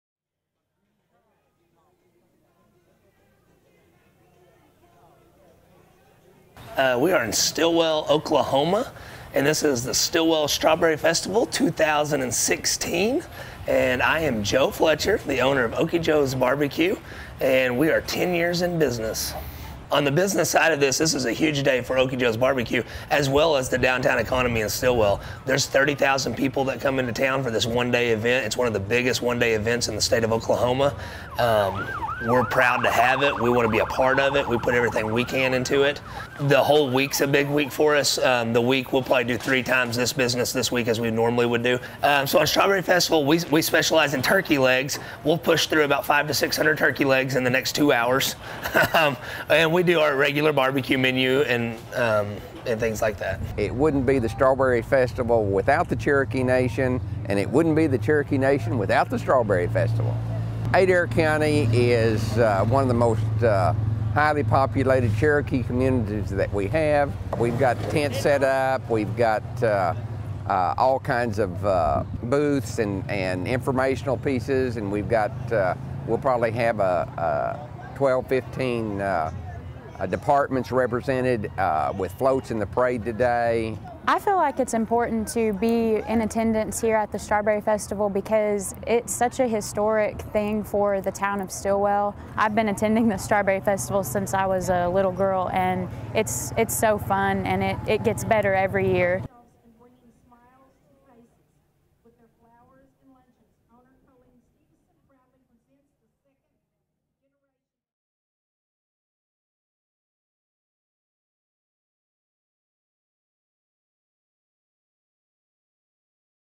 Stilwell holds 69th annual Strawberry Festival
The event included a strawberry competition, carnival rides and street vendors.